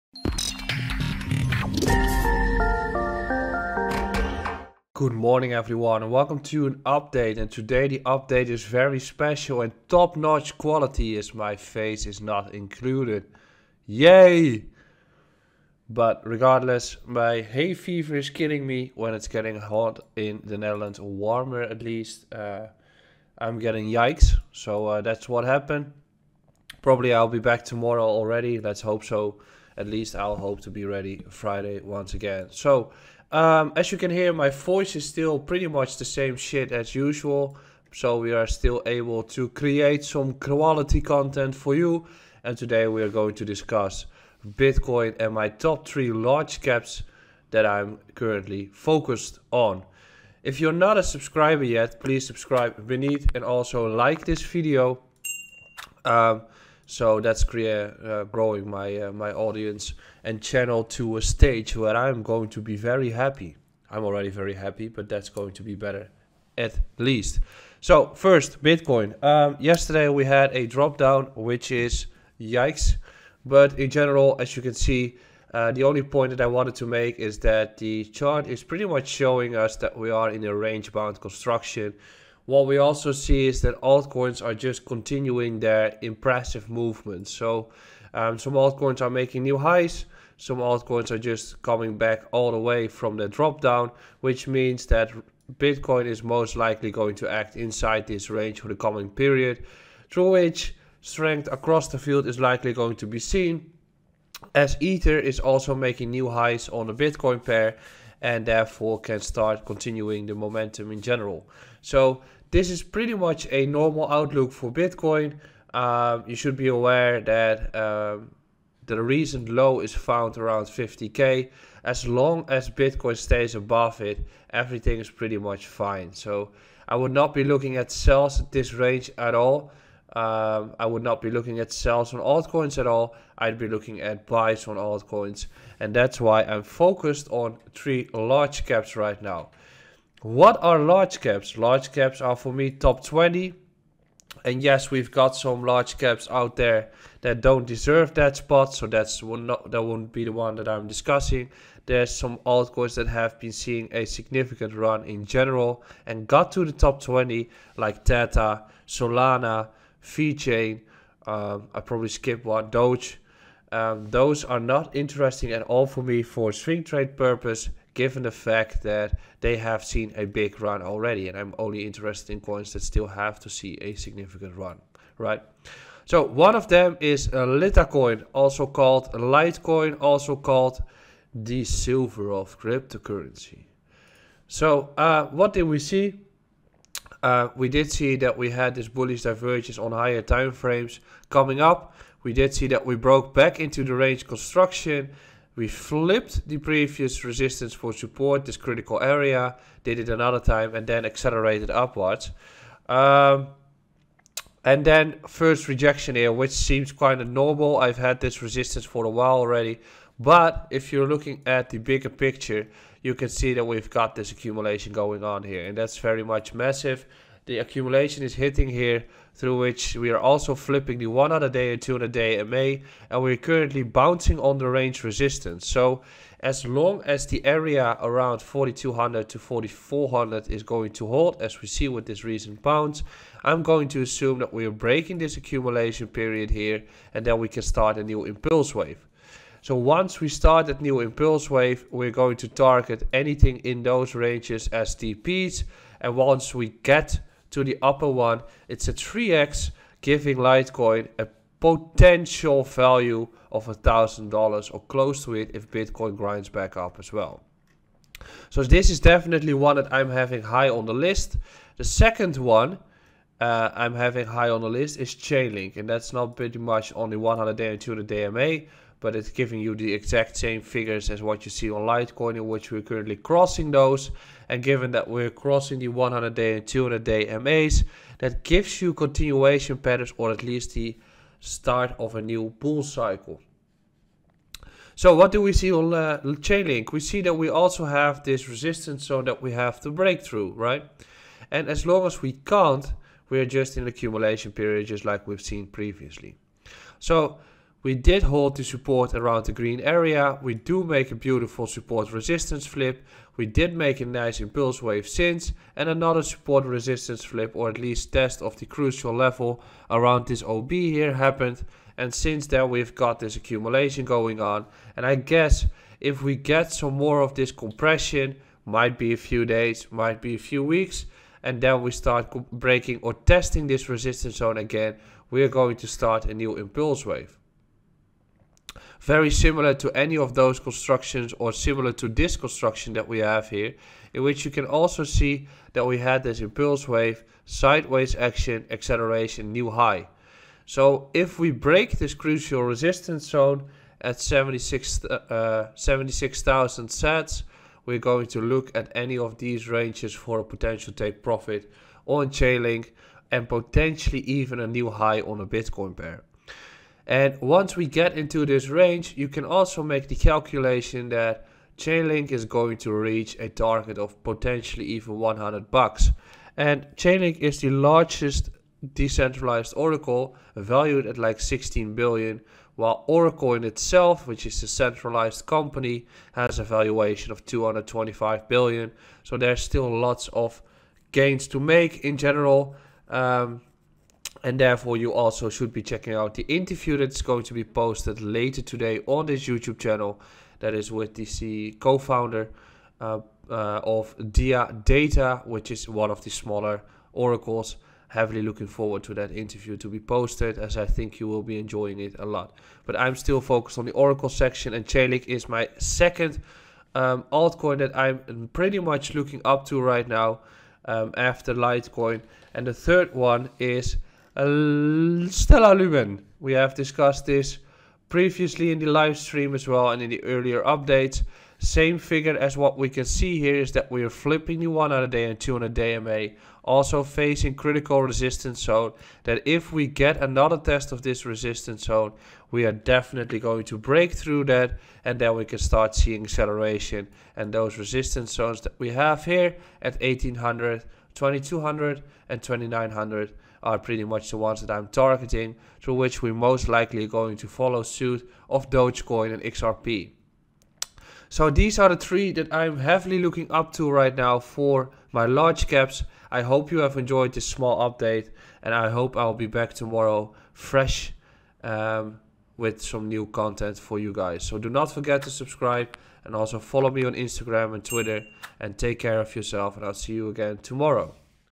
🚀 TOP 3 LARGE CAP CRYPTO COINS TO WATCH 👀
In this video I'm going to discuss my top 3 large cp cryptocurrencies that I'm watching right now. Are you invested in any of those? Are there other large caps that you are keeping an eye on?

0:00 Intro
1:08 BTC/USD
2:57 Coin 1
4:21 Coin 2
6:53 Coin 3

Where do I buy my Bitcoin and crypto? My primary exchange is Bitvavo.